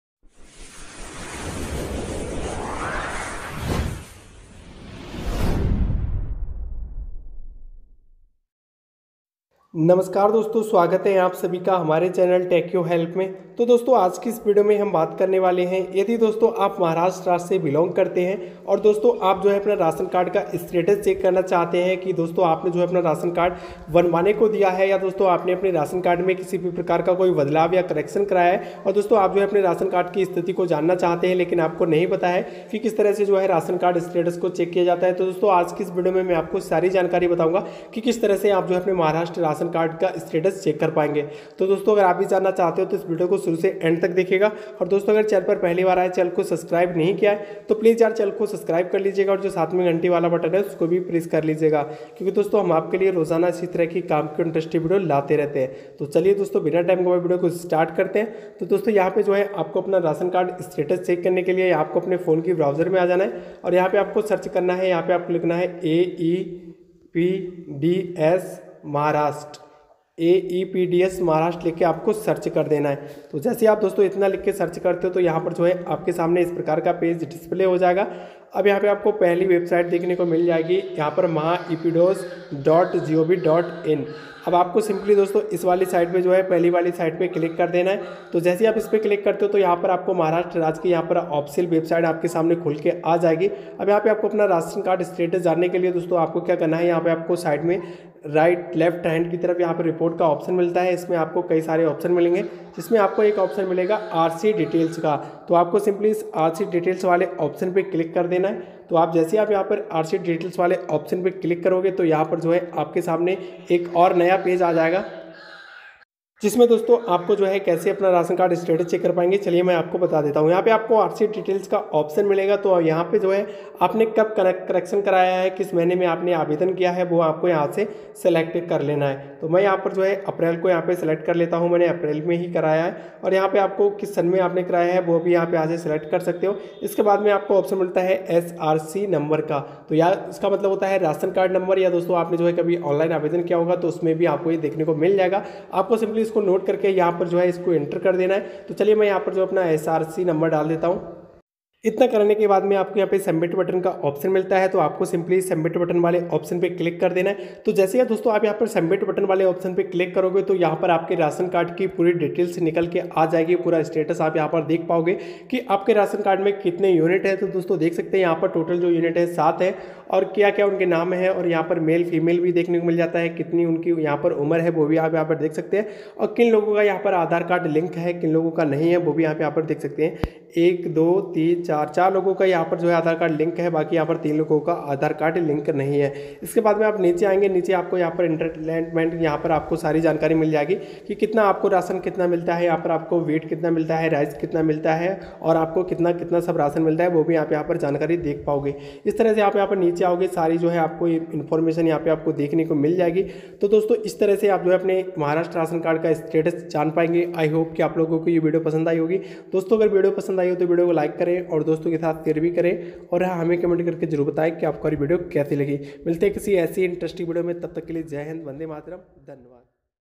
maharashtra ration card status check kaise kare | how to check ration Card status in maharashtra
Hello doston swagt h aap sabhi ka hamare youtube channel me.aaj ki is video me hmne apko bataya ki maharashtra ration card status check kaise kare . doston agr aap maharashtra ration card status check karna chahte h to is video ko pura dekhiye.